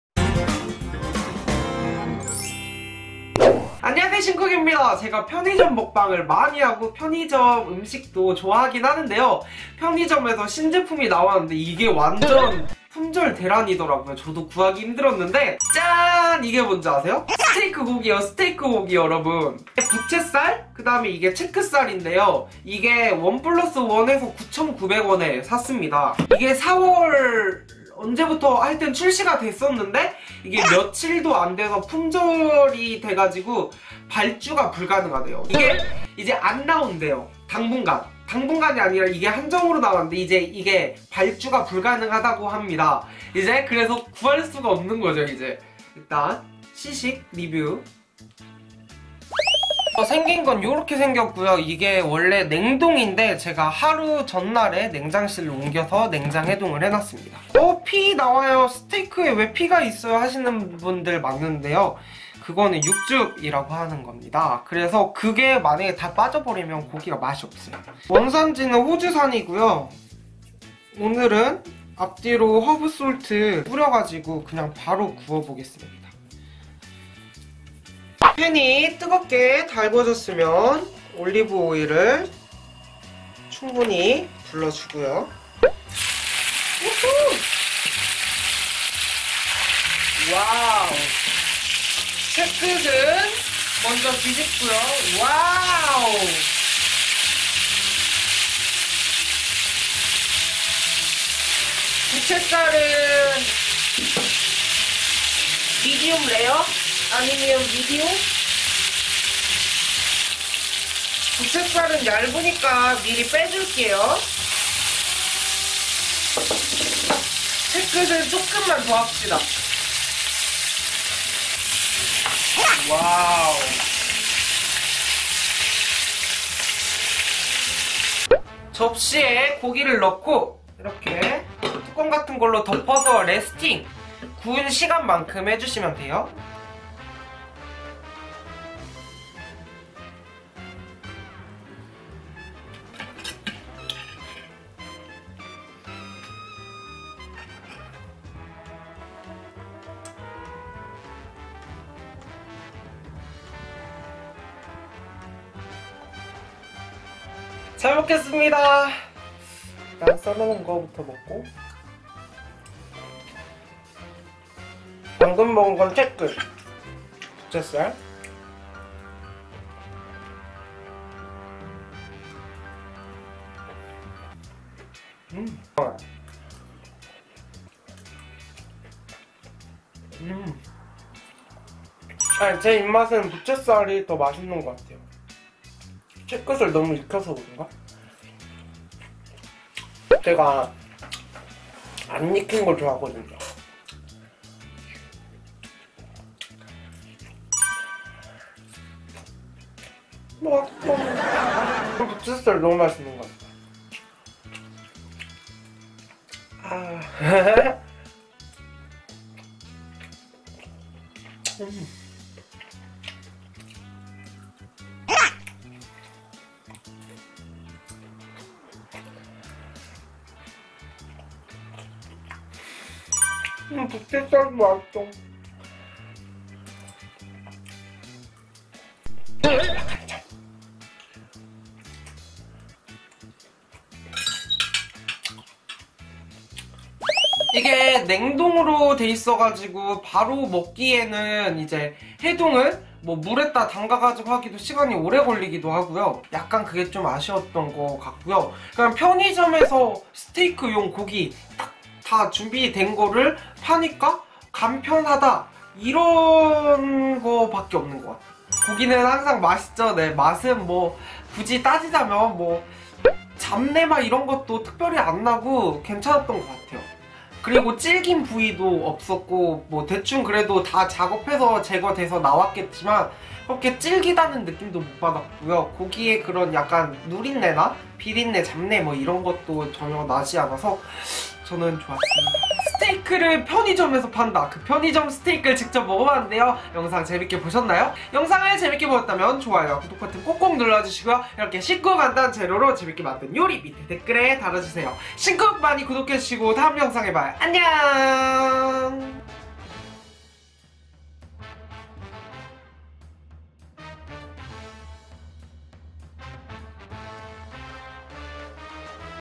품절대란!! 편의점스테이크 시식리뷰!! 2개에 9900원!! 편의점 고기가 이런맛이 나...??(부채살,채끝살) SINCOOK-신쿡
신쿡의 일상,소통은 인스타그램 팔로우!

이젠 구할수도 없는 편의점 스테이크를 마지막날 사왔습니다!!
없는게 없는 편의점이네요!! 저렴이 신제품 스테이크!!